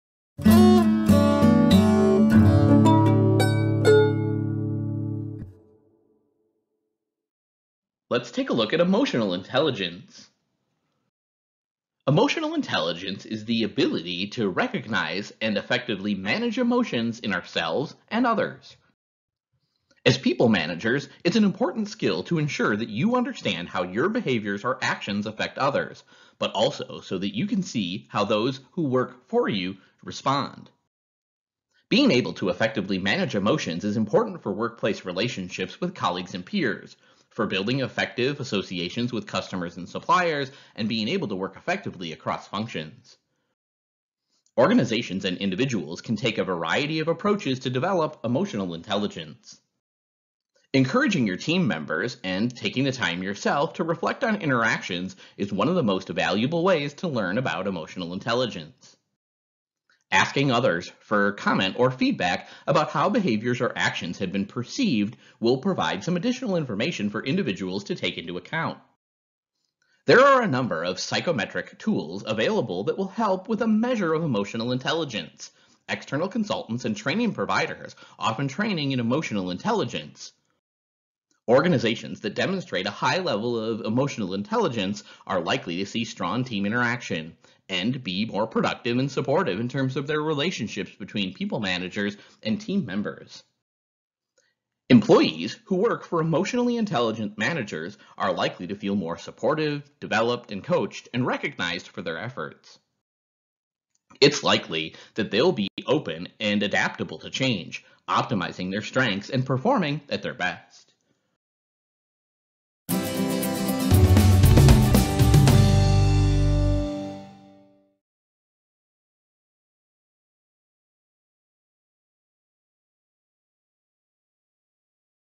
Emotional Intelligence
Emotional intelligence is the ability to recognize and effectively manage emotions in ourselves and others. As a people manager, this is an important skill to ensure that you understand how your behaviors or actions affect others, but also so that you can see how those who work for you respond. Being able to effectively manage emotions is important for workplace relationships with colleagues and peers, for building effective associations with customers and suppliers, and being able to work effectively across functions.

Organizations and individuals can take a variety of approaches to develop emotional intelligence: Encouraging your team members, and taking the time yourself, to reflect on interactions is one of the most valuable ways to learn about emotional intelligence. Asking others for comment or feedback about how behaviors or actions have been perceived will provide some additional information for individuals to take into account. There are a number of psychometric tools available that will help with a measure of emotional intelligence. External consultants and training providers offer training in emotional intelligence.

Organizations that demonstrate a high level of emotional intelligence are likely to see strong team interactions, and productive and supportive relationships between people managers and their team members. Employees who work for emotionally intelligent managers are likely to feel more supported, developed and coached, and recognized for their efforts. It is likely that they will be open and adaptable to change, optimizing their strengths and performing at their best.